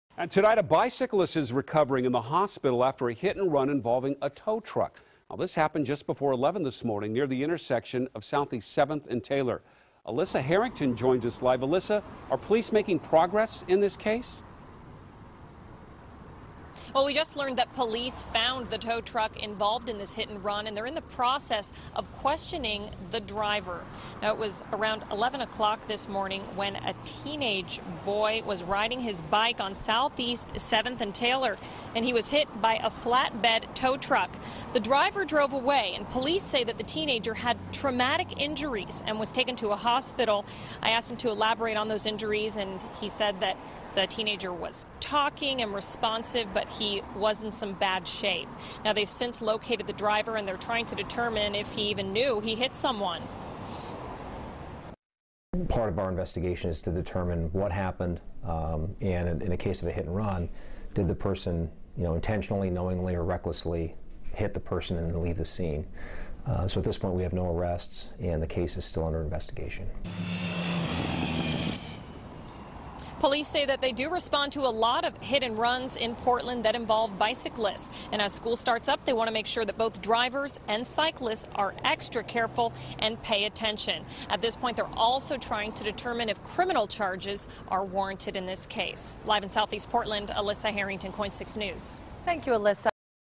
Tow truck driver found in bike hit-run
A tow truck driver who allegedly hit a bicyclist and fled the scene in Southeast Portland Wednesday morning was located later in the afternoon by police, but no arrests have been made.